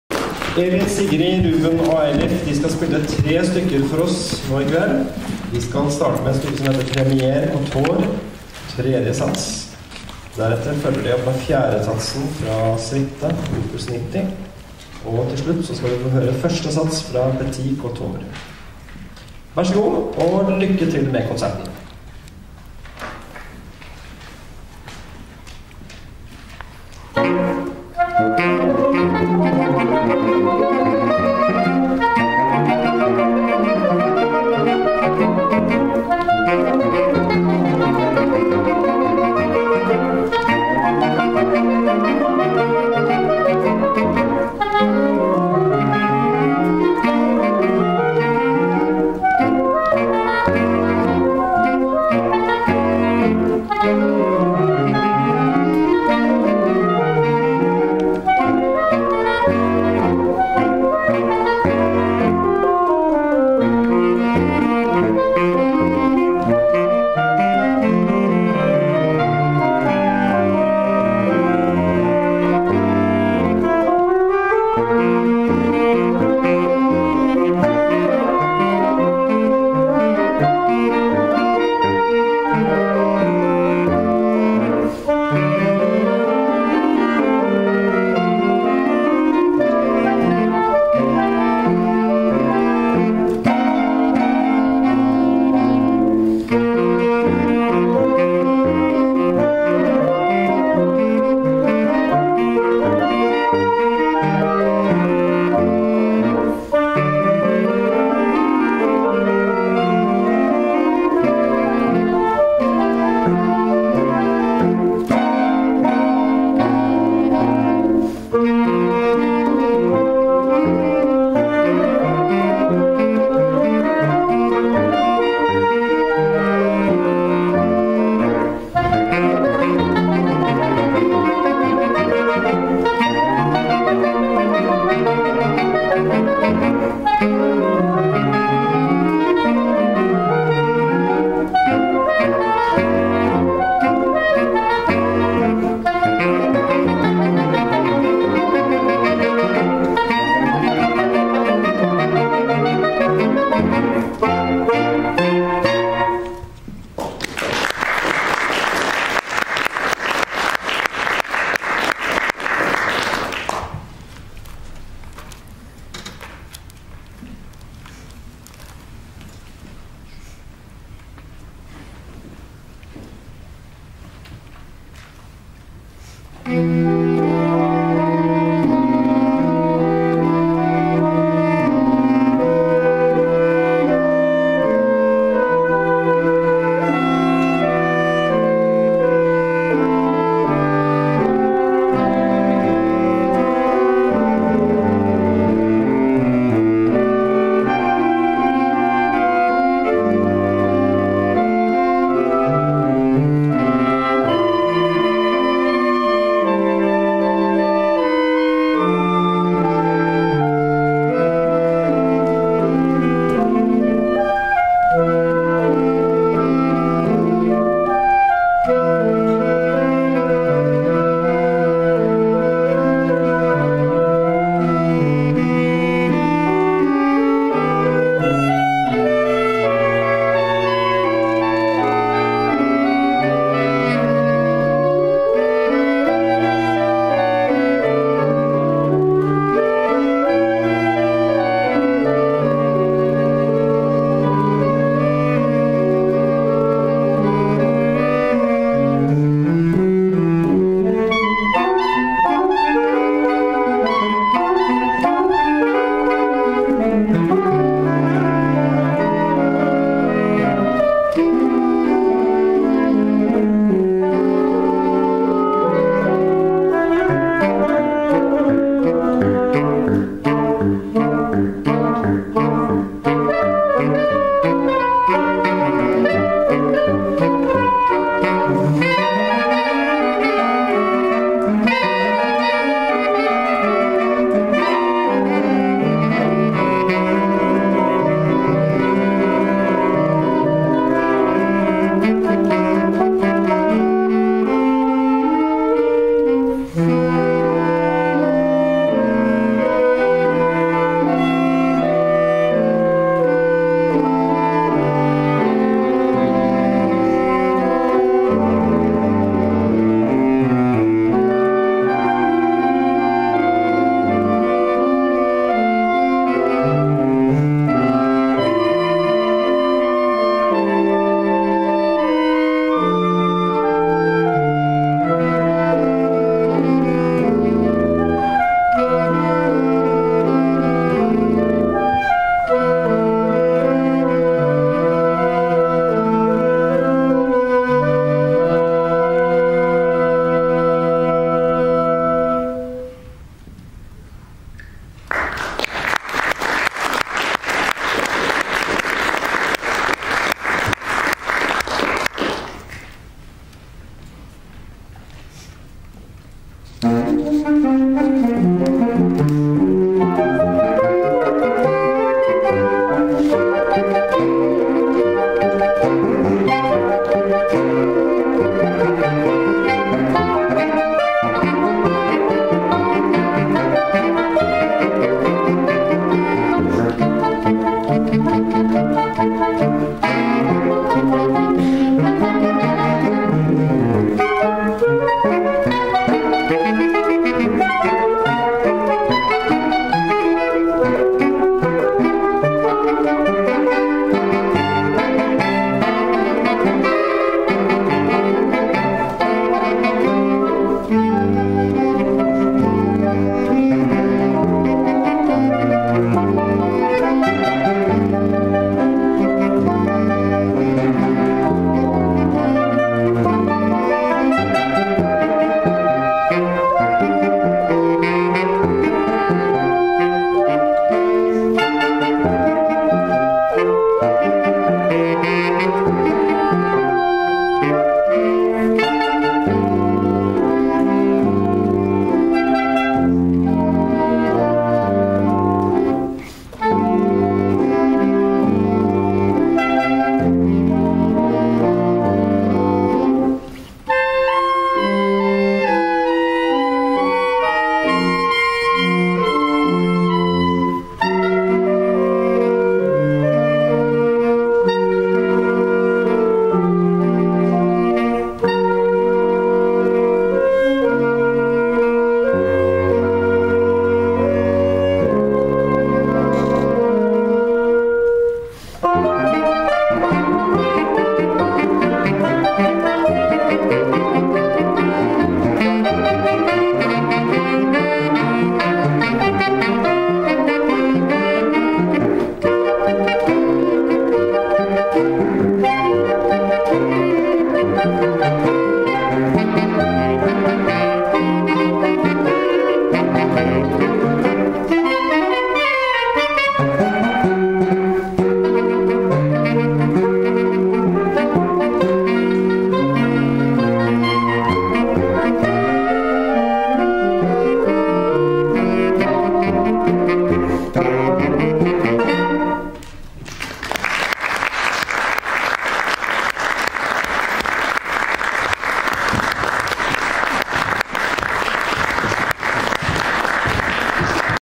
Jean Baptiste Singelee. Jean Absil. Jean Francaix
Elin Fossum(15) Sigrid Nese(13) Ruben Horn(13) Eilif Syverud(13) Concours regional de musique de chambre de Oslo 2012. Teacher: Jan Petter Lilja Bye